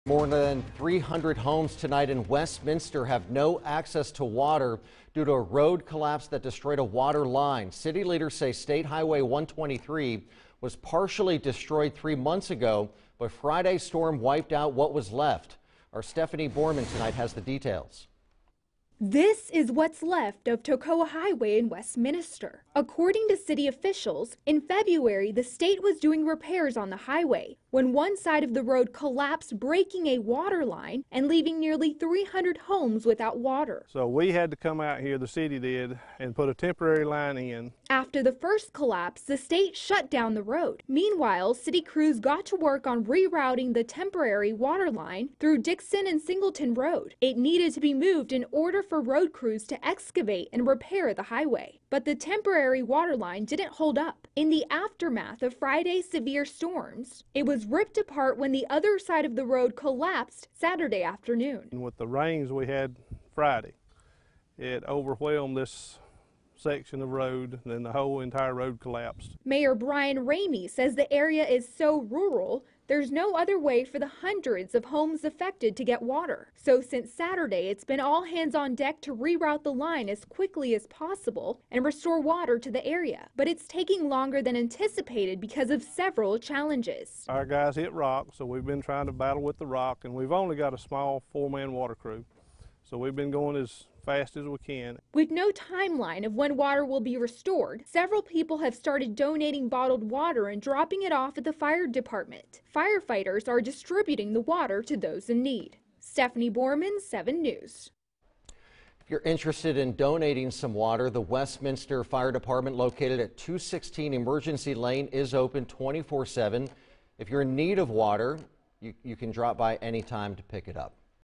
Hundreds without water after Hwy. 123 washes out in Westminster
Around 300 households are without water after a water line broke when storms caused a washout along Highway 123 in Westminster.